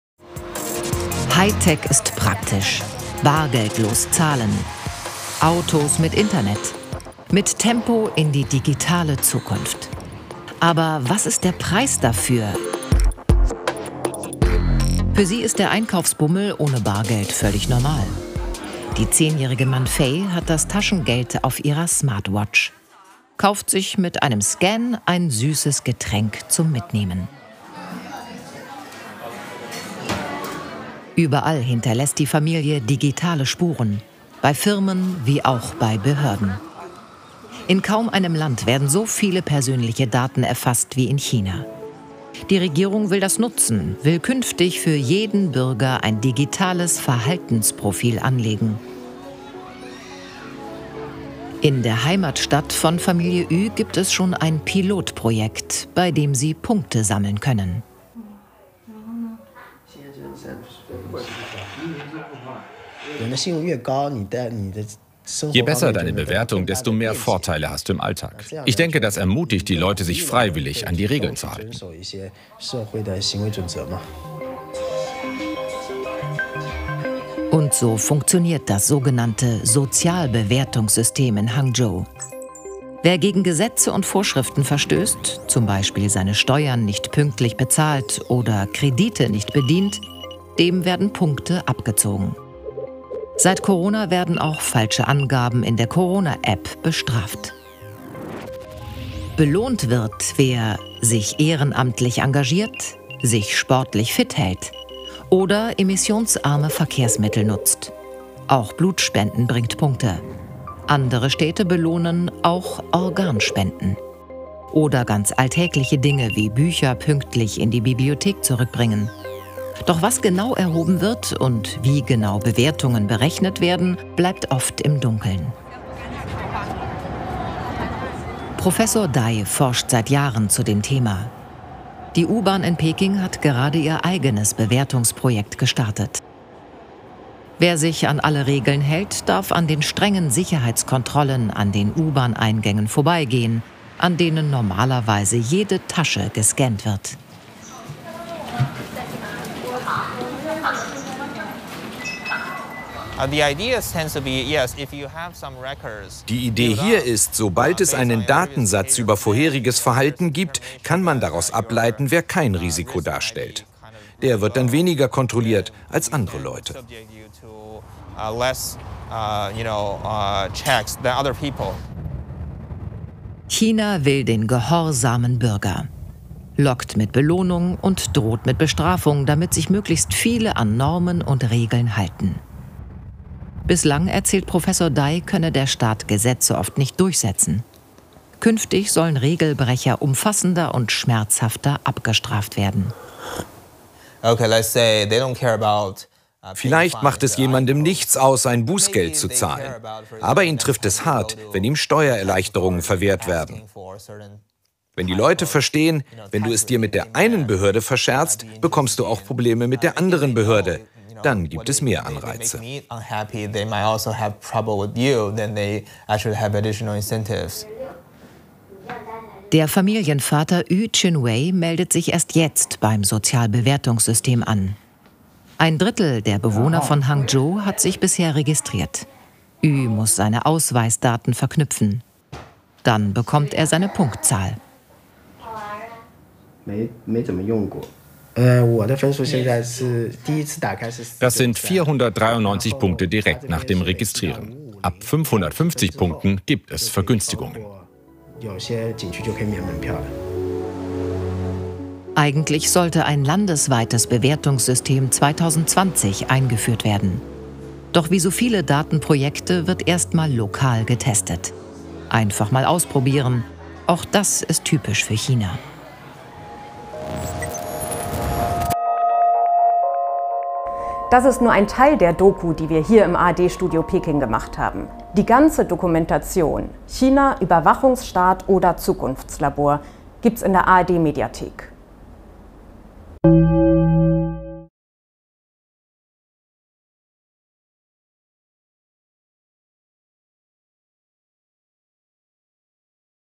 Social Scoring - so läuft Chinas Bewertungssystem | China: Überwachungsstaat oder Zukunftslabor?
Wer mitmacht und sich an alle Regeln hält, bekommt online einen Discount – dank eines Sozialbewertungssystems. Wo sich Deutsche fürchten, sehen laut Umfragen die meisten Chinesen Vorteile: Wenn Algorithmen jeden Bürger an seinem Gesicht, seiner Sprache und sogar an seinem Gang lokalisieren können, haben Raser und Rüpel, Betrüger und Verbrecher keine Chance.

China baut ein digitales Überwachungssystem auf, das Kritiker als "das ehrgeizigste orwellsche Vorhaben der Menschheitsgeschichte" bezeichnen. Dabei entsteht eine "schöne neue Welt", in der sich die Bürger Annehmlichkeiten, Vergnügungen, Konsum und damit fast unmerklich auch der totalen Kontrolle hingeben: